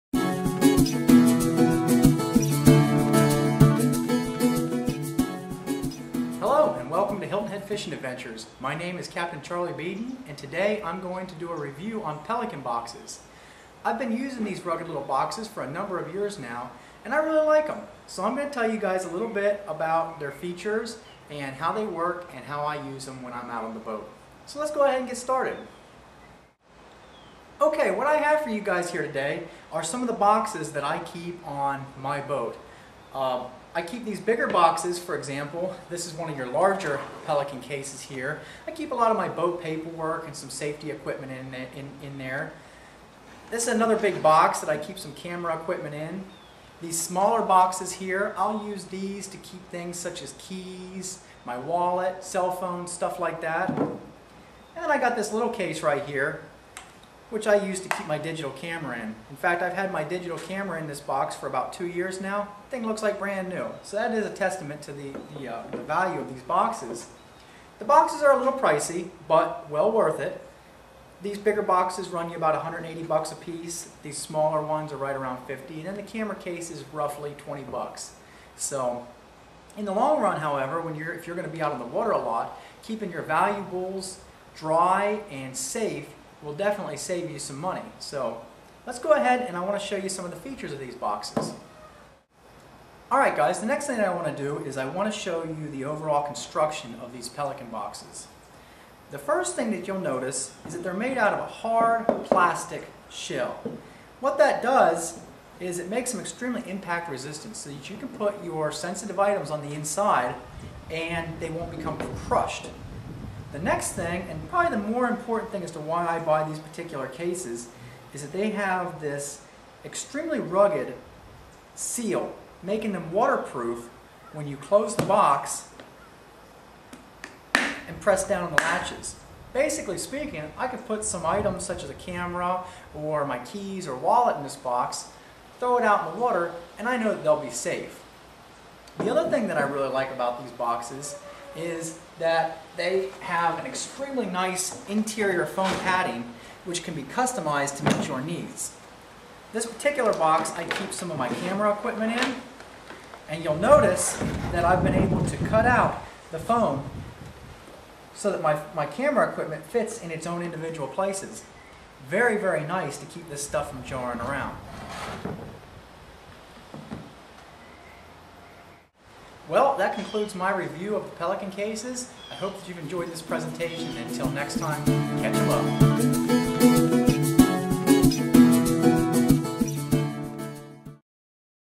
Pelican Cases - Review for Boating and Fishing
This is a review of Pelican Cases. I use these rugged boxes on a daily basis while out boating and fishing to keep my valuables safe and secure. In this review I will show you several different boxes, their construction and how you can use them to protect your belongings.